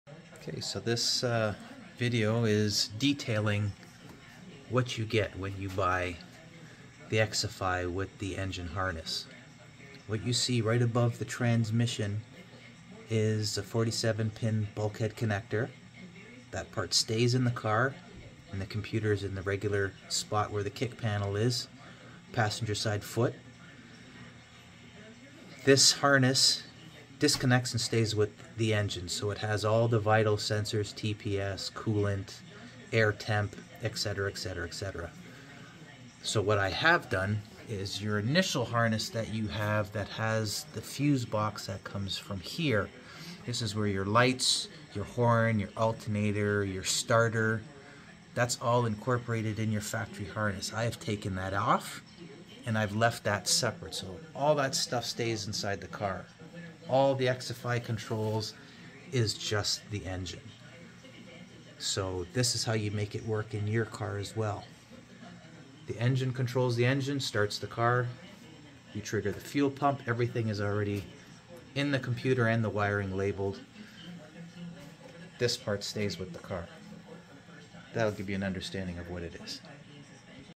XFI engine harness explained while the engine is out of the car.
This is the XFI system explained and detailing it's function.